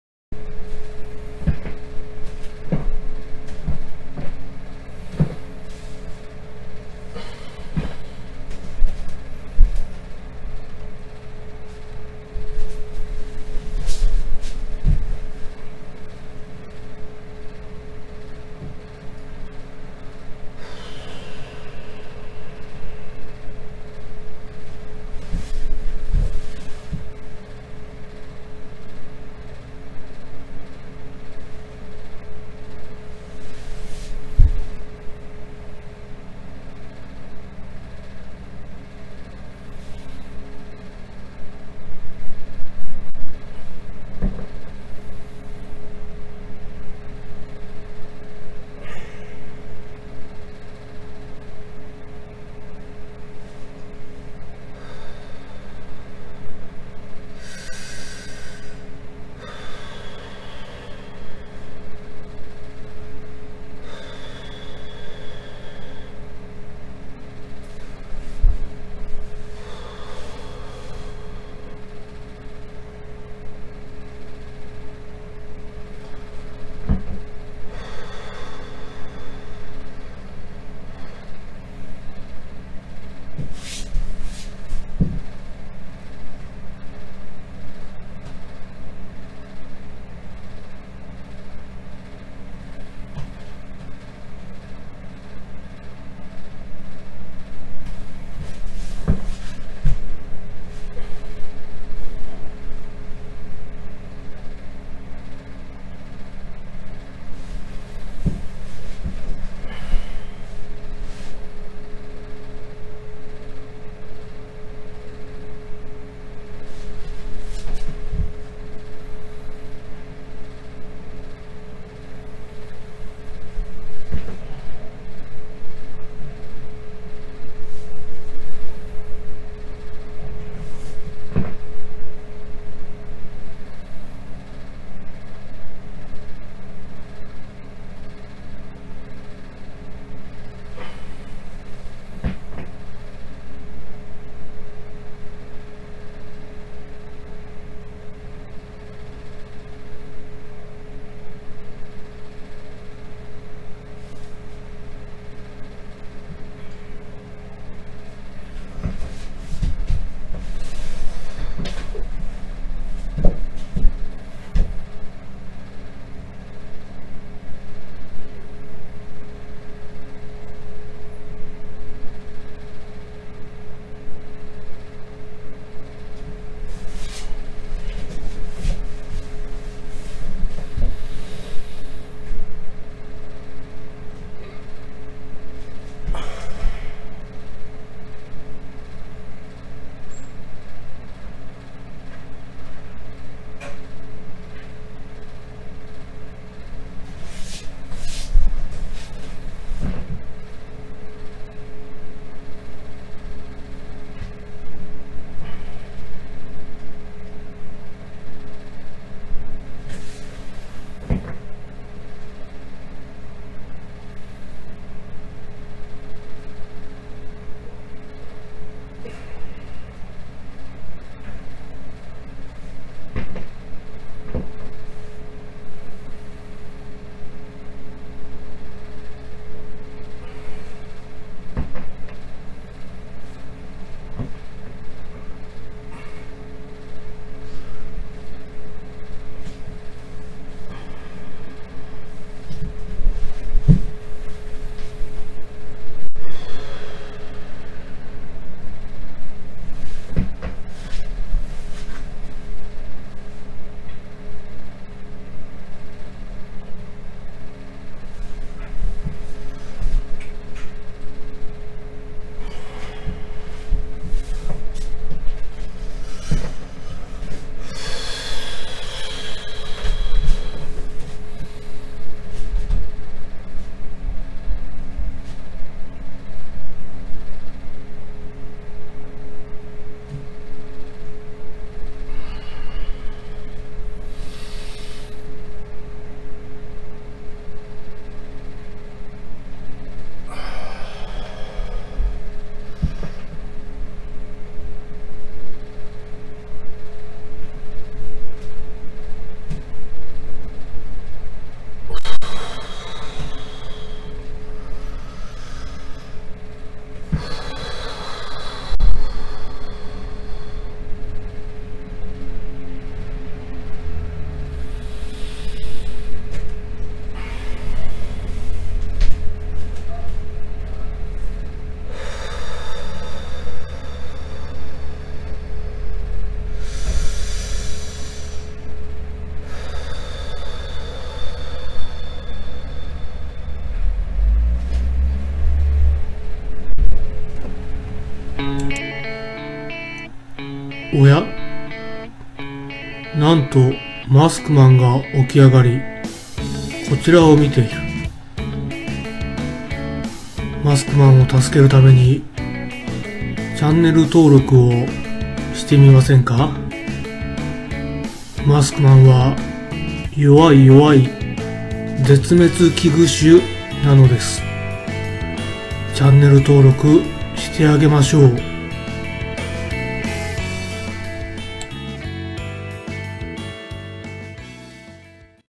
無職の瞑想「ストレッチするも運動不足で体が硬い」2018年8月9日
猛暑が熱暑かった頃。最近は、瞑想してない。迷走しとるけど。